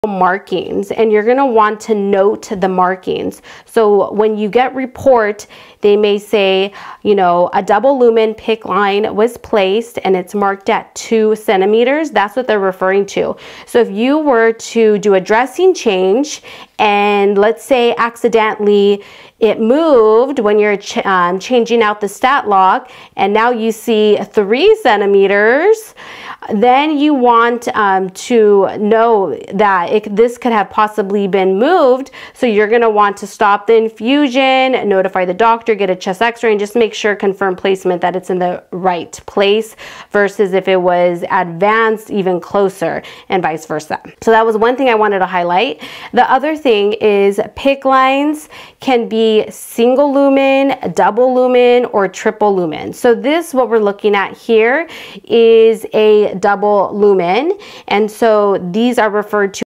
PICC LINE REVIEW IN 60 SECONDS
Welcome back in this video I will go over a general review of your PICC lines from anatomical placement, with a review of the parts of a PICC line and what's included in your dressing and MORE.

Don't forget to checkout my links below, and ill catch you on the next on

Here is the link to find out more about me and what I offer: